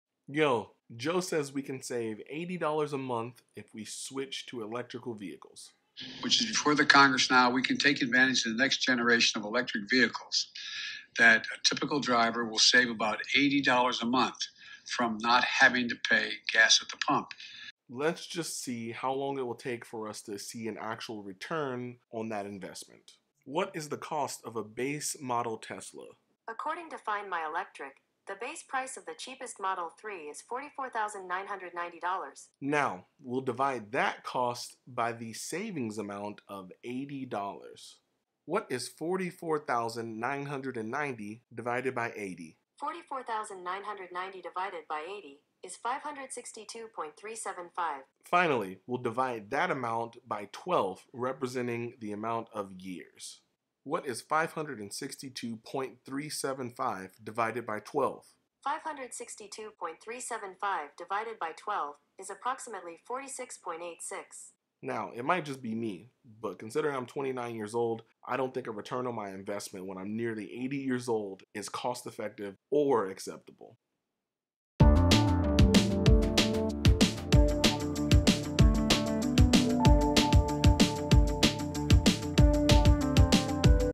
Get an electric car....
This video highlights the return on investment (ROI) of getting an electric vehicle if you are poor.